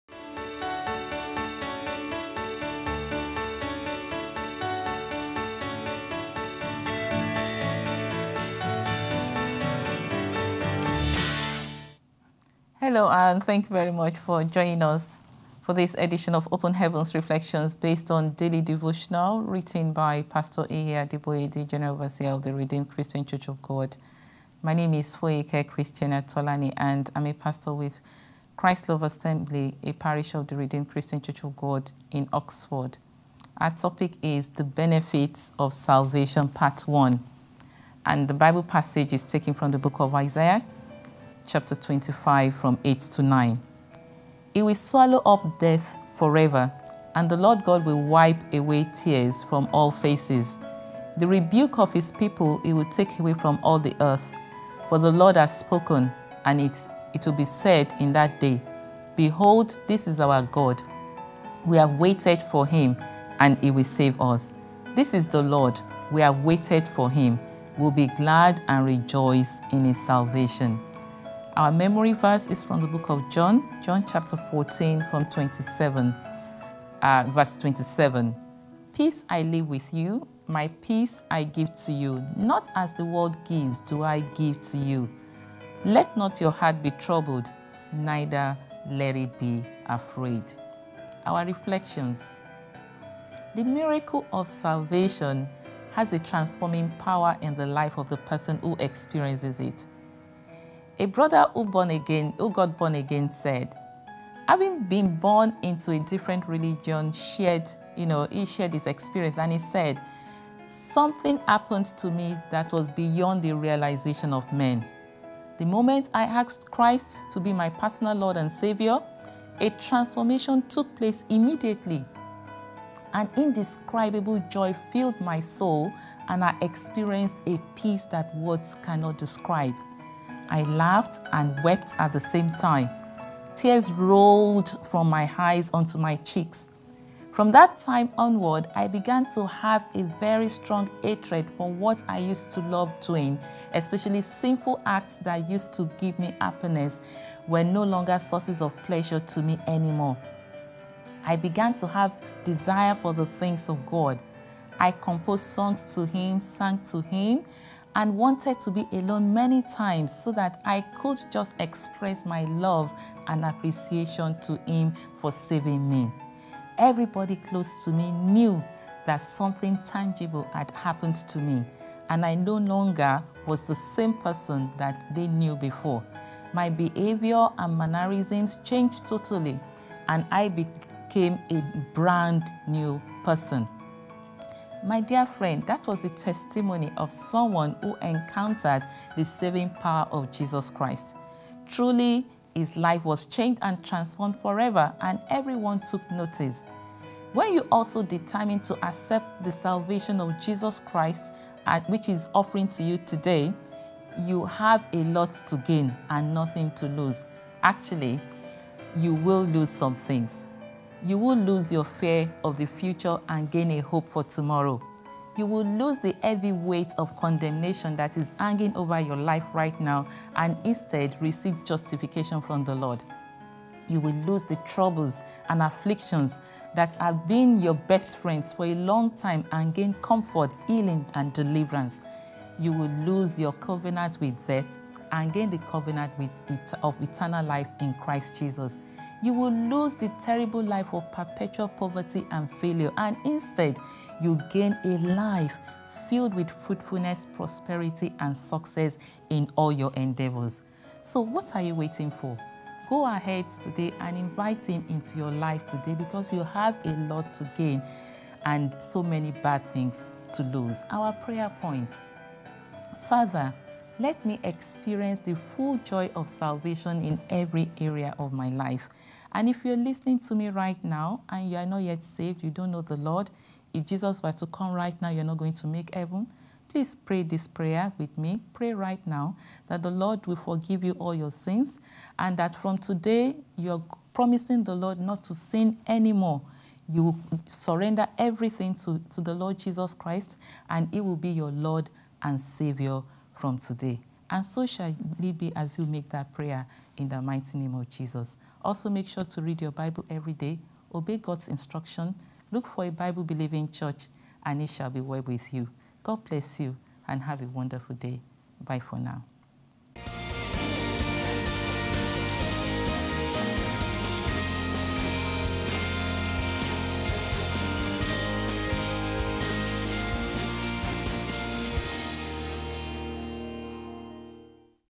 Open Heavens Reflections 26 April 2015 - The Benefits of Salvation 1
Reflections by Pastor Foyeke Christiana Tolani, RCCG, Christ's Love Assembly, Oxford, UK based on the Open Heaven Daily Guide written by Pastor E. A. Adeboye, The General Overseer of The Redeemed Christian Church of God.